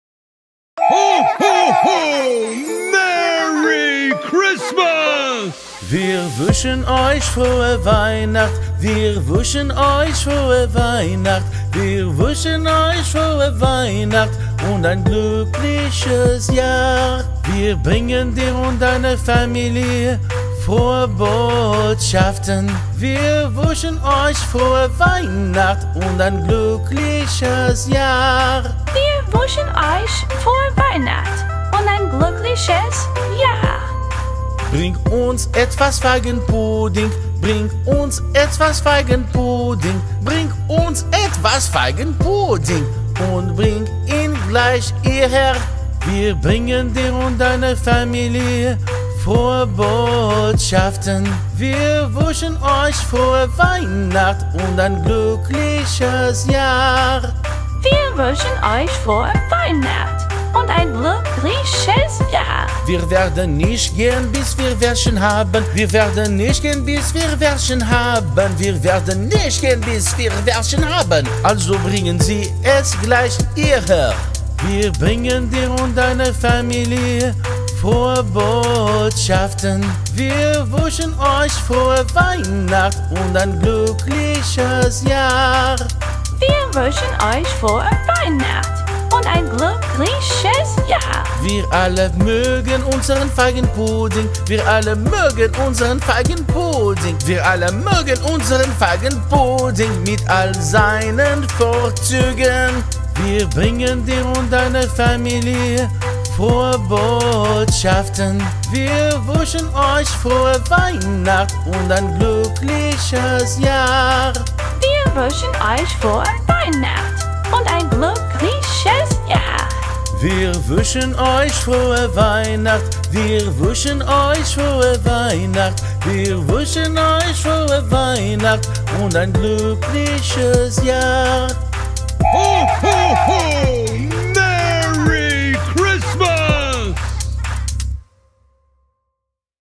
Frohe Weihnachten Beliebtes Weihnachtsgedicht für Vorschulkinder auf Deutsch
Weihnachten Kinderreime ist eine entzückende Sammlung festlicher Lieder und Reime, die kleinen Kindern die Magie der Weihnachtszeit näherbringen. Diese Reime sind voller eingängiger Melodien, fröhlicher Texte und bezaubernder Weihnachtsthemen und eignen sich perfekt zum Mitsingen für Kleinkinder und Vorschulkinder. Von bimmelnden Glöckchen bis zu verschneiten Tagen führen diese Kinderreime Kinder in die Freude weihnachtlicher Traditionen, Winterwunderländer und Feiertagsspaß ein. Christmas Nursery Rhymes ist ideal für Familientreffen, Aktivitäten im Klassenzimmer oder gemütliche Momente zu Hause. Es erfüllt die Luft mit weihnachtlicher Stimmung und macht es zu einem unvergesslichen und lehrreichen Erlebnis für die Kleinen, während sie die Weihnachtszeit feiern!